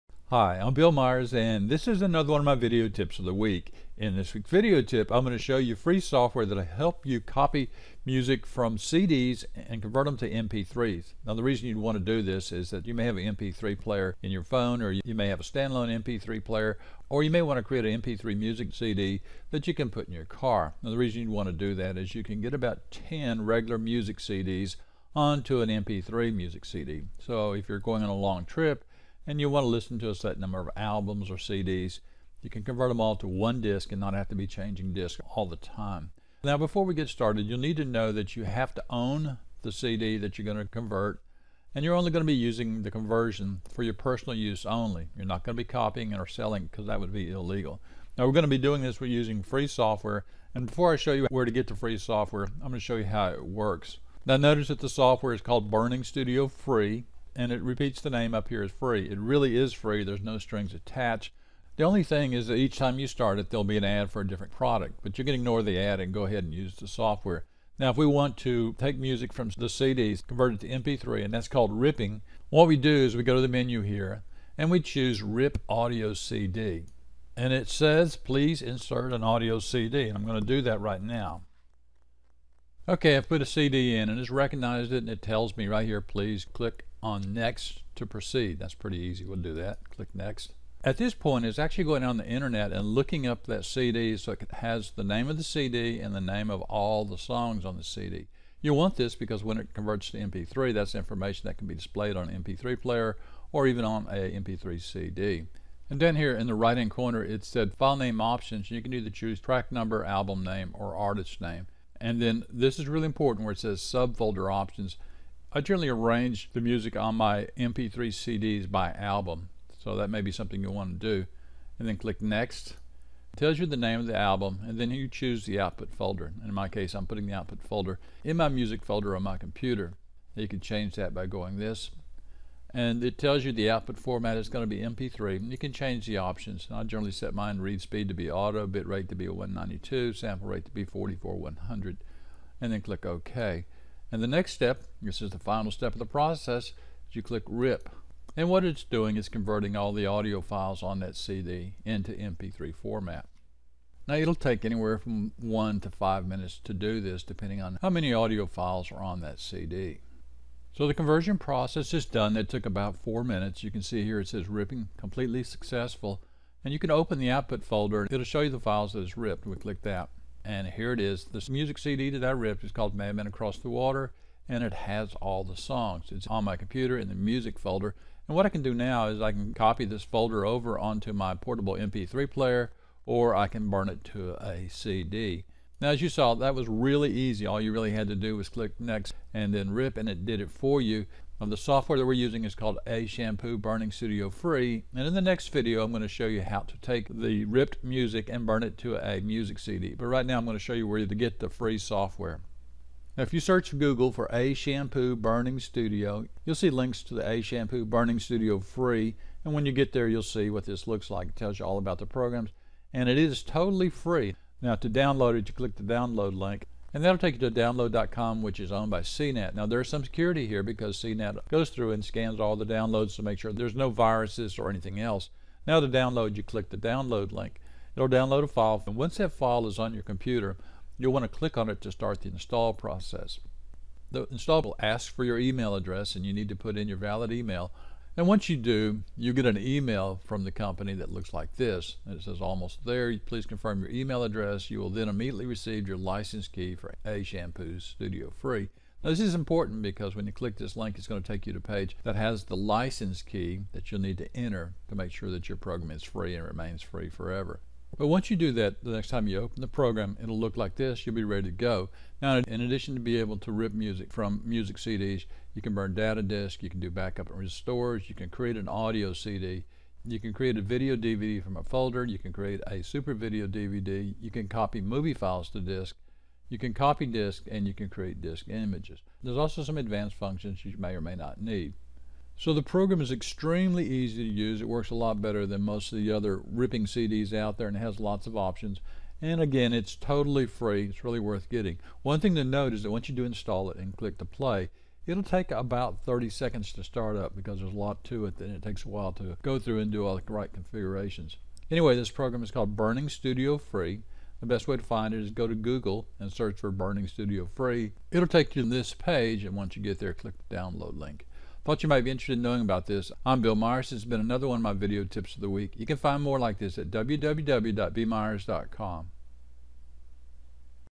Convert music on CDs to MP3 - Rip using free BurningStudio
Here's the quick and easy way to convert all your music CDs into MP3 files using free Burning Studio Free. find it